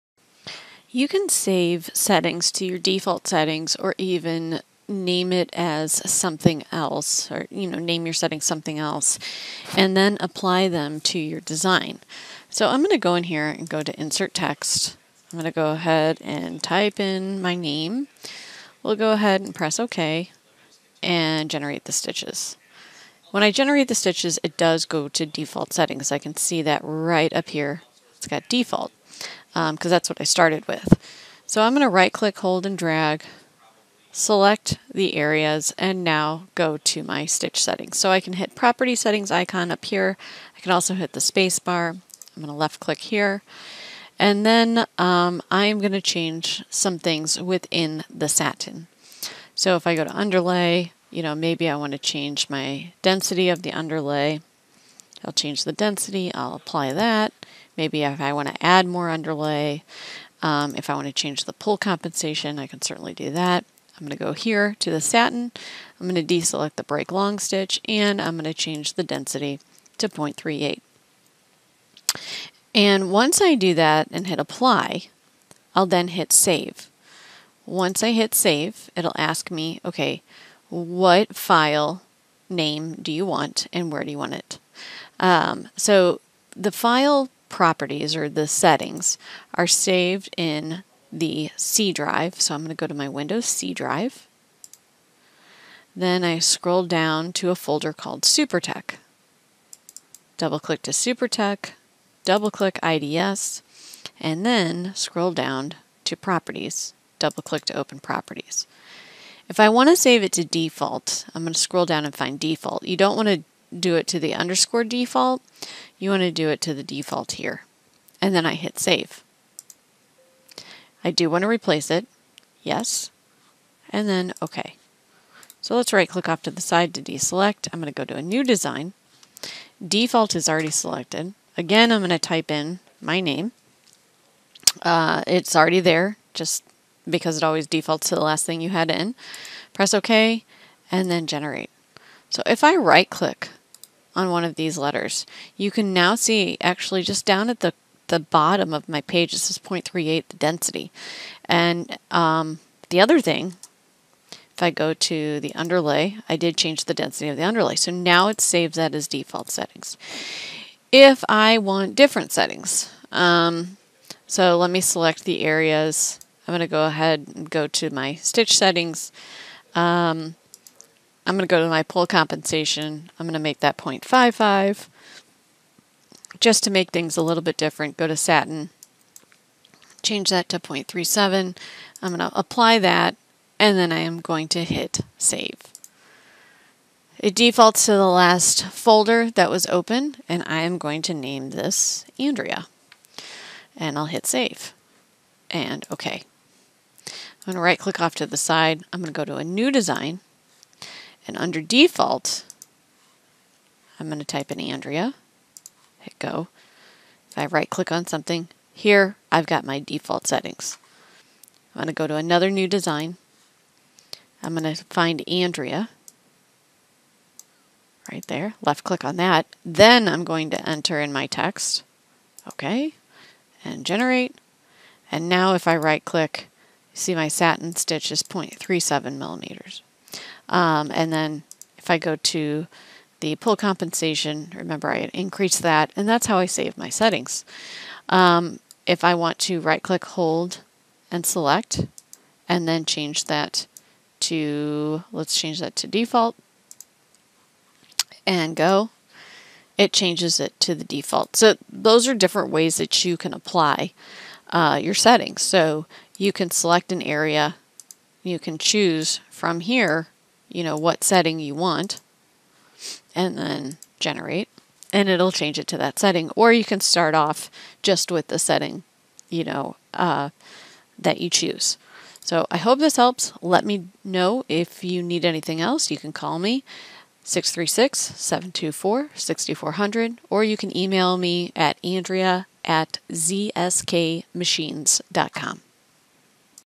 Saving Settings in IDS
Save your favorite settings in IDS as default or save as a special name. Just apply them as you want!